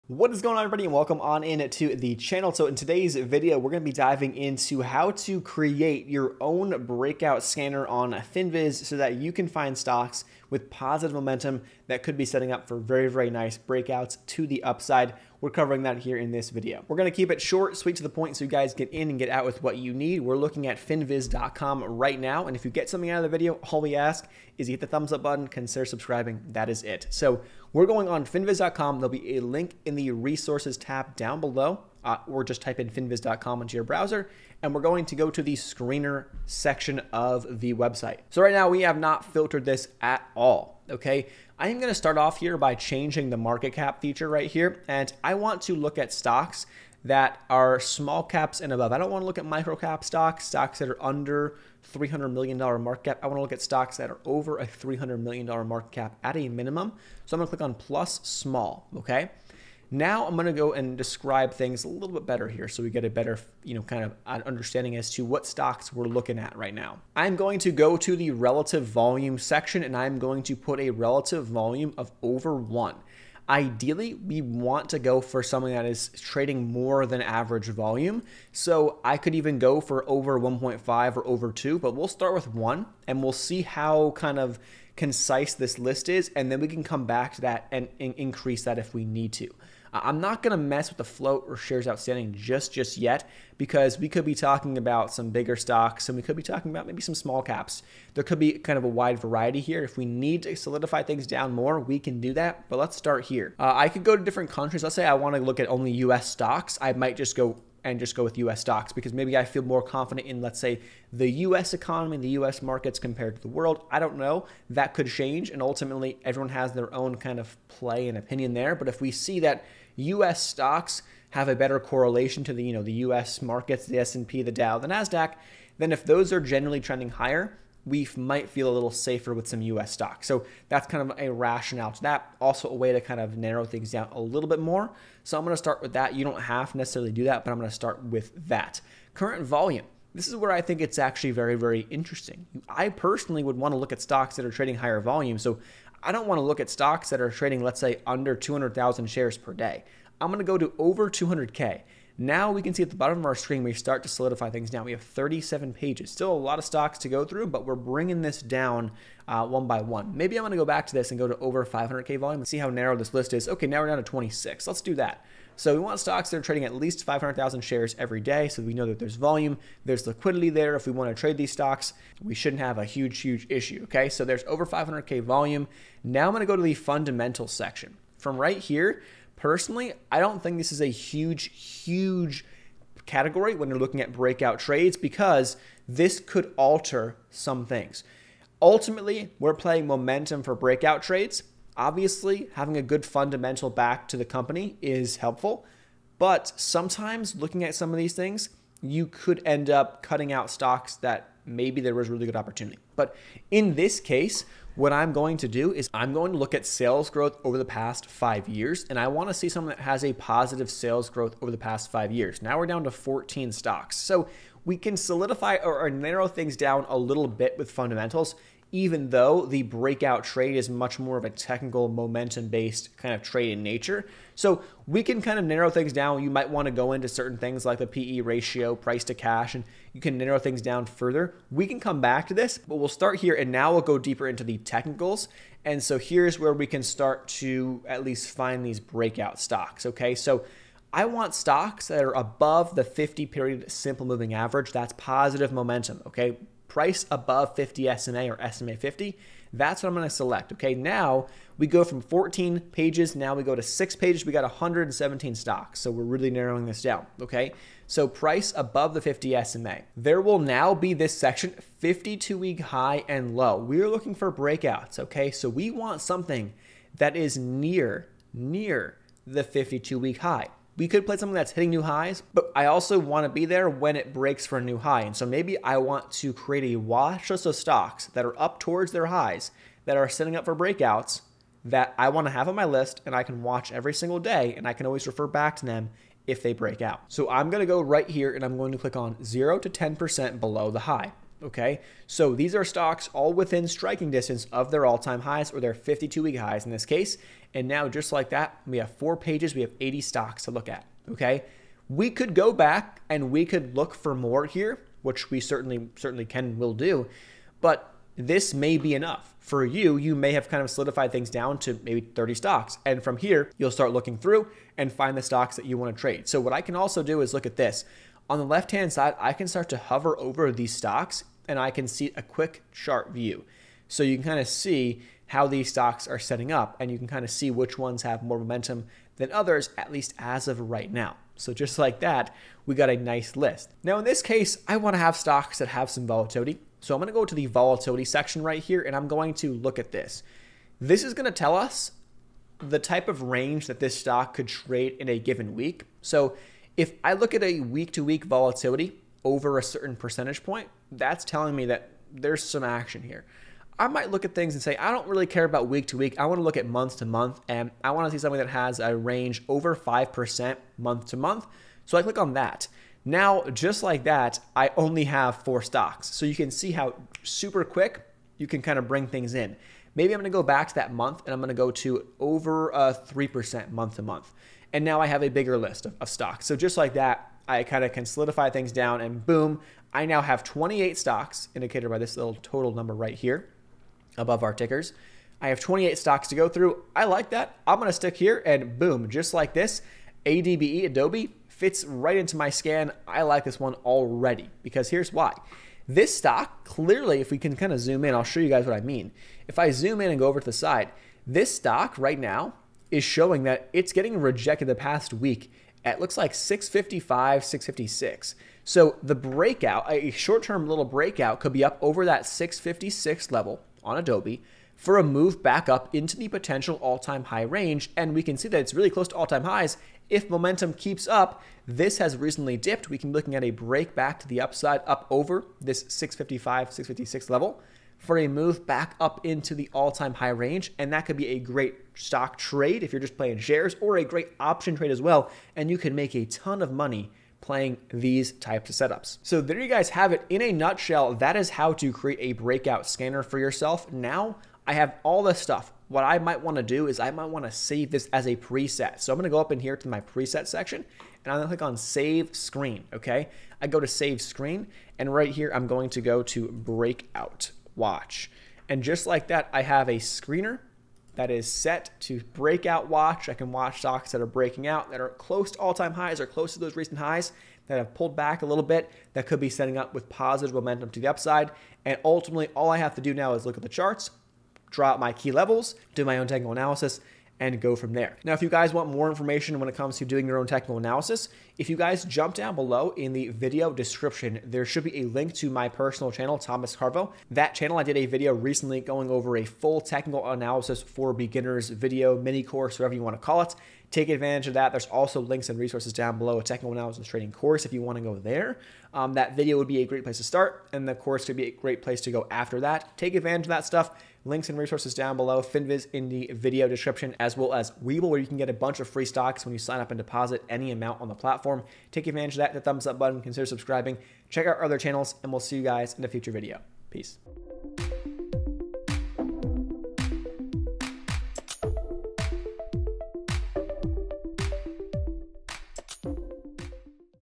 How To Create A Winning Finviz Breakout Scanner | Find Stocks Before They Breakout
In this video we are covering exactly how you can create your own Finviz breakout stock scanner step by step. We cover what you need to know key parameters, how to save, edit and more!